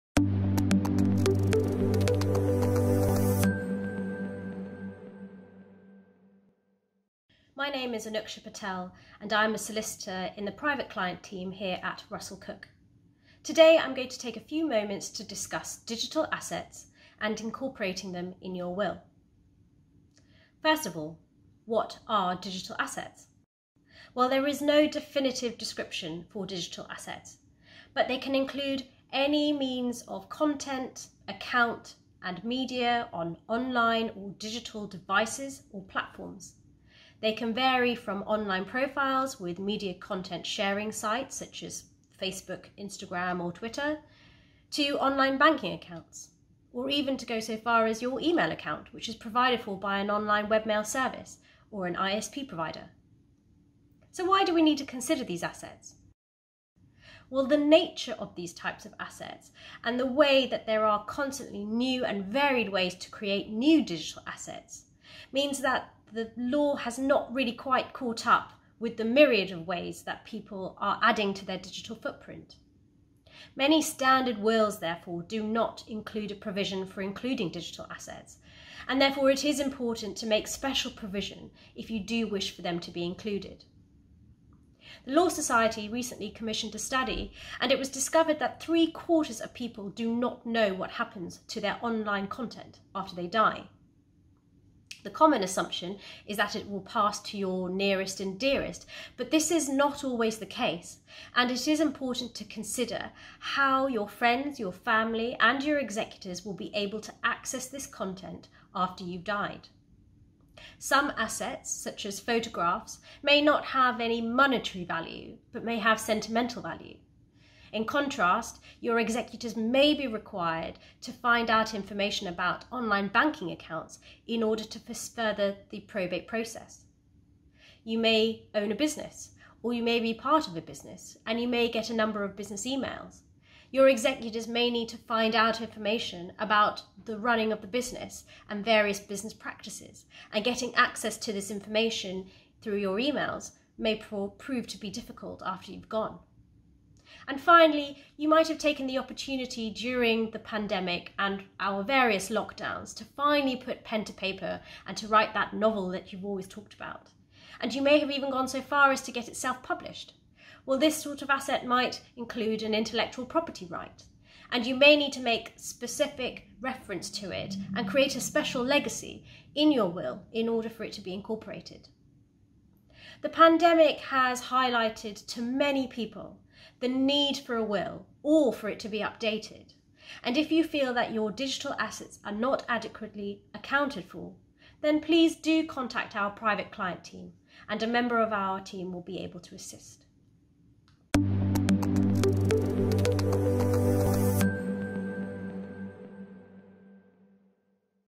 Digital assets and incorporating them in your will
Russell-Cooke solicitors video. Anouksha Patel discusses digital assets and incorporating them in your will. Anouksha advises on wills, lasting powers of attorney, probates, trusts, estate planning and cross-border issues.